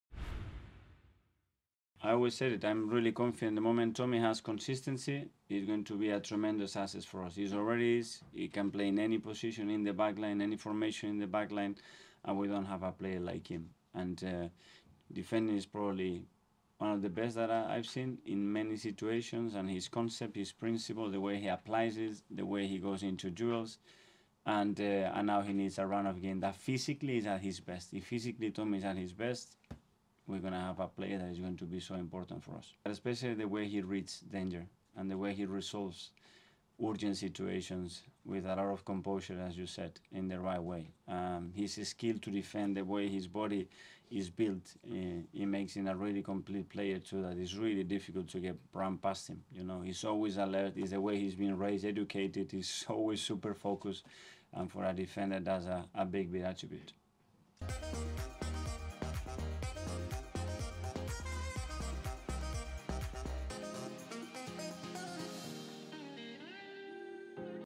【大絶賛】「冨安は最高の選手」アーセナル × ボーンマス｜アーセナル アルテタ監督 試合前インタビュー｜日英字幕｜2023年9月29日
23/24シーズン プレミアリーグ第7節  アーセナル × ボーンマス
試合前のアーセナル アルテタ監督のインタビュー。

ミッドウィークのカラバオカップでPOTMにも選ばれ、コンディションが良い
冨安選手について前日会見で質問が飛んでいたのでピックアップ。
ここまで言うならプレータイム与えてくれ、、と思いますが、
アルテタ監督は会見では誰にでもこれくらいの良い評価はコメントで残すので
大きく状況は変わらなさそう、、、頑張って欲しい！
⚫︎Pick Up Word/Phrase
・principle：原則
・composure：冷静
・attribute：特性

このチャンネルを通して、欧州サッカーへの理解を深めると共に、みなさまの英語学習の一助になれば幸いです。